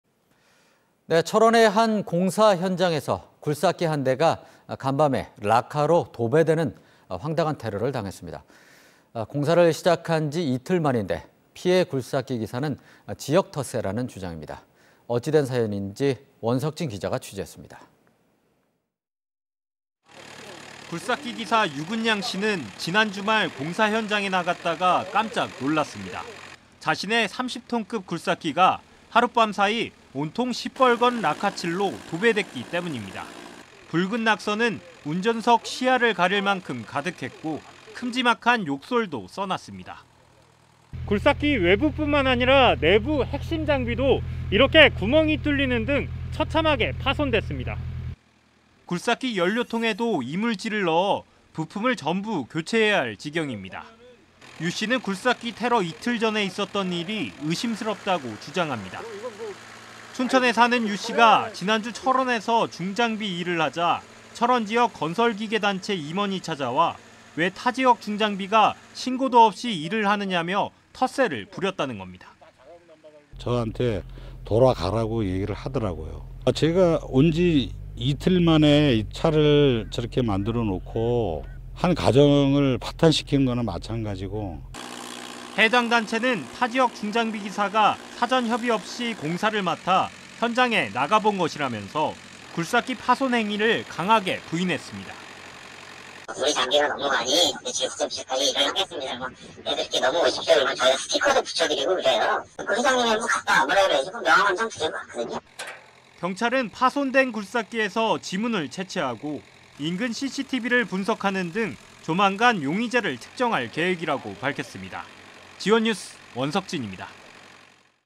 [G1뉴스] 타지역 공사 갔다가 굴삭기 테러..'텃세 탓?'
[앵커]
철원의 한 공사 현장에서 굴삭기 한 대가 간밤에 락카로 도배되는 황당한 테러를 당했습니다.

공사를 시작한 지 이틀 만인데, 피해 굴삭기 기사는 '지역 텃세'라는 주장입니다.
어찌된 사연인지, 원석진 기자가 취재했습니다.

[리포터]
굴삭기 기사 유근양 씨는 지난 주말 공사현장에 나갔다가 깜짝 놀랐습니다.

자신의 30톤급 굴삭기가 하룻밤 사이 온통 시뻘건 락카칠로 도배됐기 때문입니다.

붉은 낙서는 운전석 시야를 가릴 만큼 가득했고, 큼지막한 욕설도 써놓았습니다.

"굴삭기 외부 뿐만 아니라 내부 핵심장비도 이렇게 구멍이 뚫리는 등 처참하게 파손됐습니다."

굴삭기 연료통에도 이물질을 넣어 부품을 전부 교체해야할 지경입니다.

유 씨는 굴삭기 테러 이틀 전에 있었던 일이 의심스럽다고 주장합니다.

춘천에 사는 유 씨가 지난주 철원에서 중장비 일을 하자,

철원지역 건설기계 단체 임원이 찾아와 '왜 타지역 중장비가 신고도 없이 일을 하느냐'며 텃세를 부렸다는 겁니다.

[인터뷰]
"저한테 돌아가라고 얘기하더라고요. 제가 온 지 이틀 만에 차를 저렇게 만들어 놓고, 한 가정을 파탄시킨 거나 마찬가지고.."

해당 단체는 타지역 중장비 기사가 사전 협의 없이 공사를 맡아 현장에 나가본 것이라면서,

굴삭기 파손행위를 강하게 부인했습니다.

"(원래는) 우리 장비가 넘어가니 며칠부터 며칠까지 일을 하겠습니다 그러면, 네 넘어오십시오. 그럼 저희가 스티커도 붙여드리고 그래요. 회장님이 한번 갖다 오라그래서 명함 한 장 드리고 왔거든요."

경찰은 파손된 굴삭기에서 지문을 채취하고, 인근 CCTV를 분석하는 등 조만간 용의자를 특정할 계획이라고 밝혔습니다.
G1뉴스 원석진입니다.